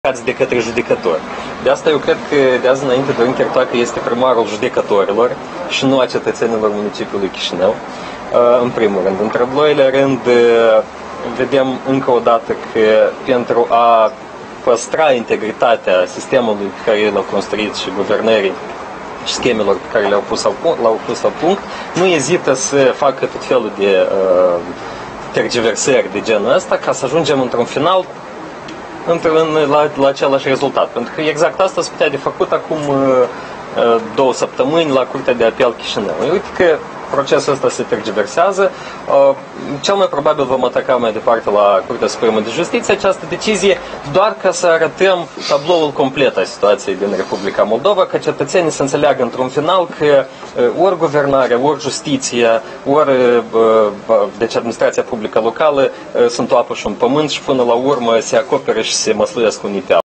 Curtea de Apel Bălți examinează recursul lui Dorin Chirtoacă împotriva deciziei ...
Victor Chironda: Dorin Chirtoacă este primarul judecătorilor!

Titlul evenimentului: Curtea de Apel Bălți examinează recursul lui Dorin Chirtoacă &#238;mpotriva deciziei Judecătoriei Centru de &#238;nregistrare a grupului de inițiativă privind organizarea referendumului de demitere a primarului